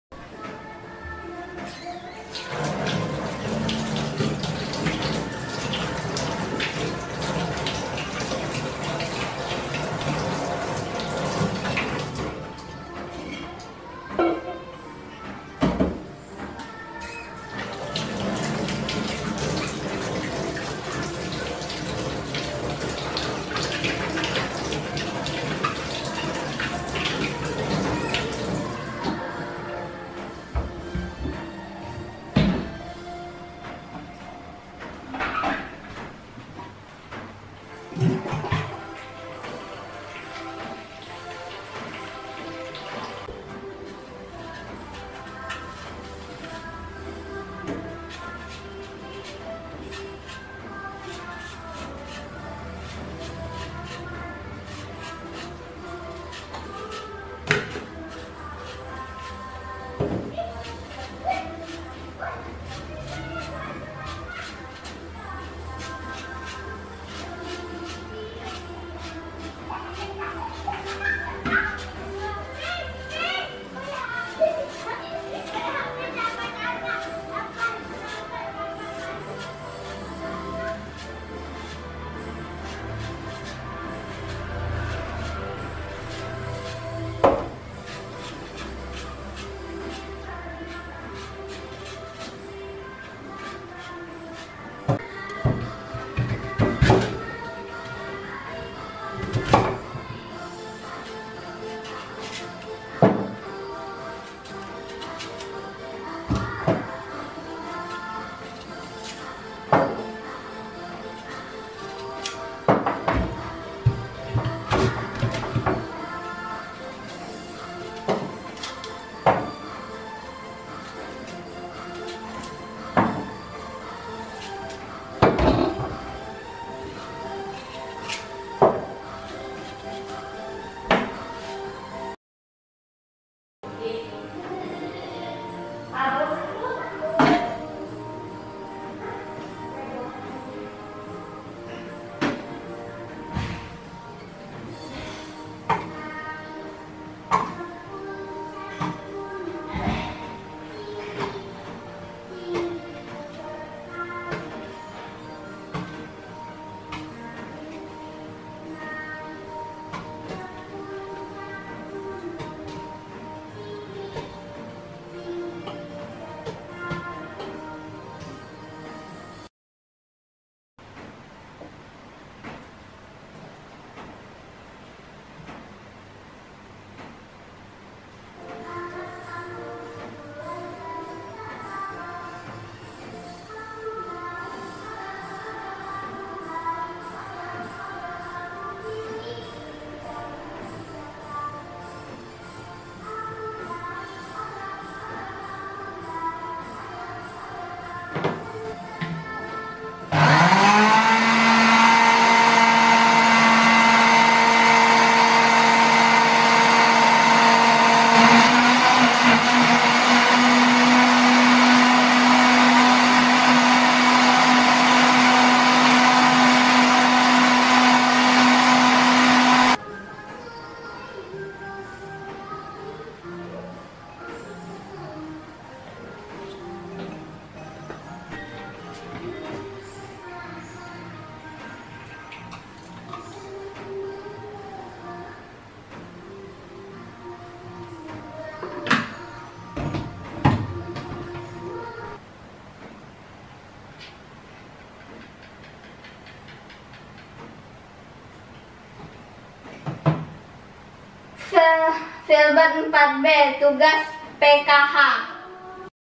Ayo Belajar Membuat Jus Apel Fun Video For Kids
Tugas sekolah membuat jus apel